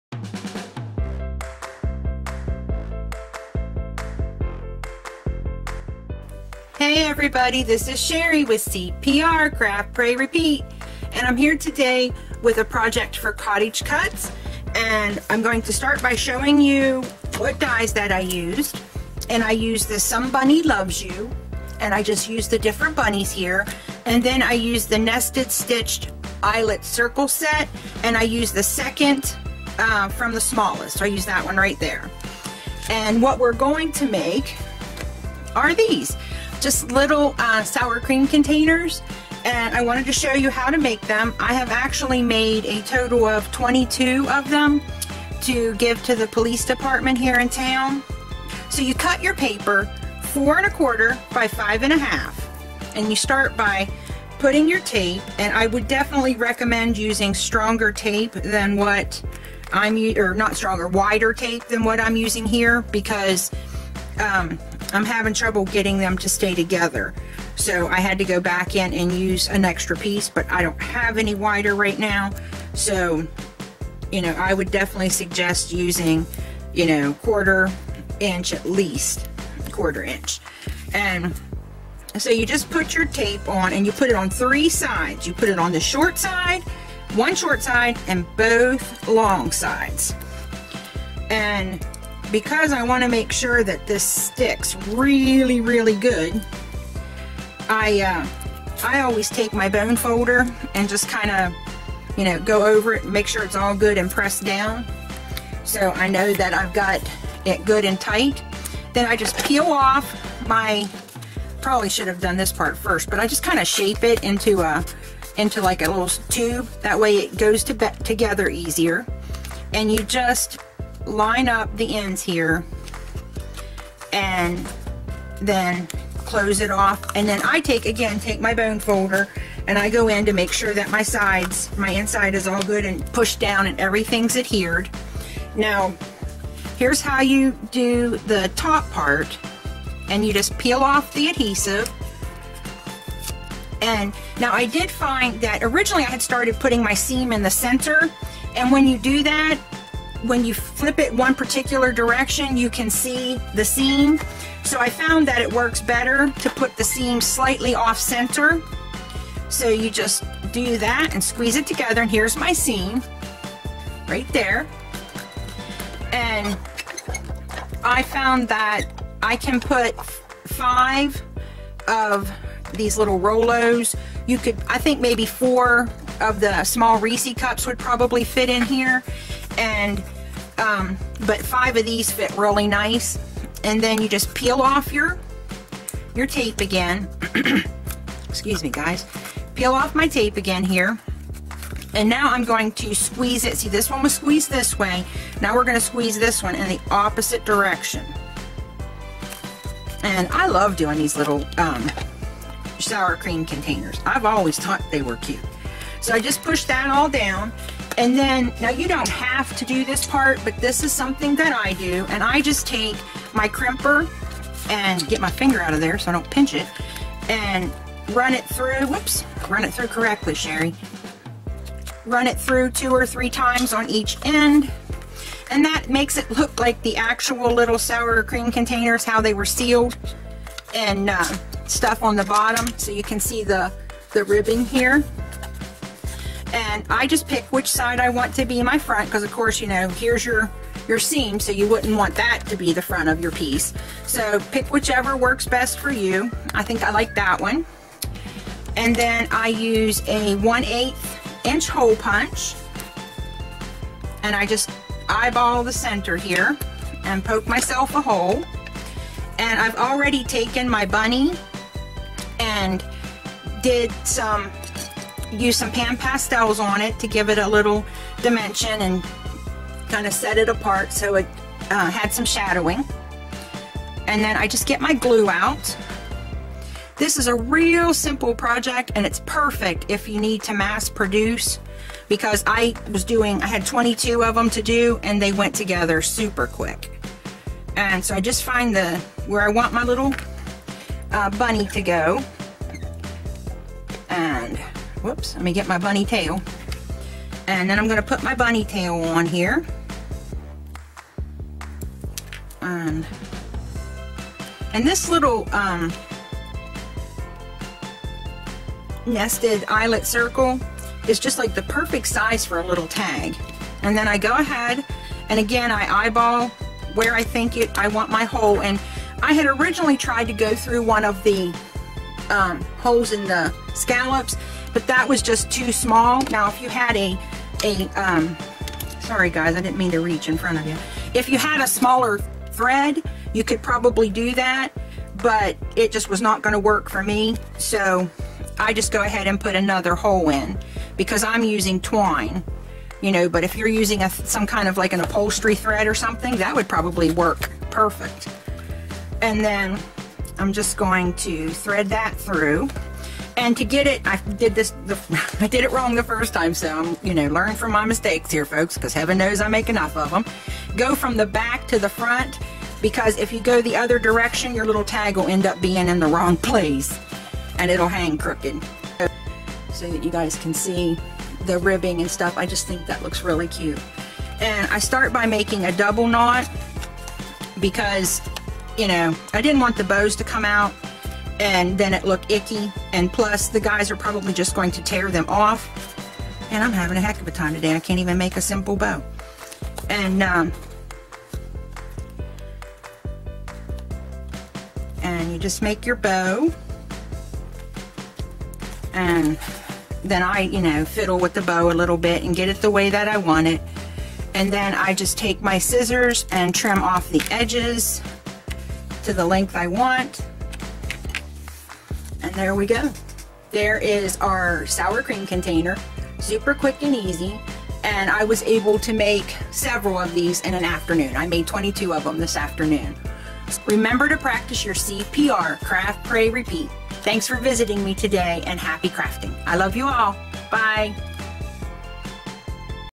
Quick and Easy Easter Treat Containers
These are so quick and easy to make, so they are awesome for projects that you need to mass produce.
Nested Stitched Eyelet Circle Set -
Remember to practice your CPR - Craft Pray Repeat. Thanks for visiting me today and happy crafting.
God Bless,
Sherry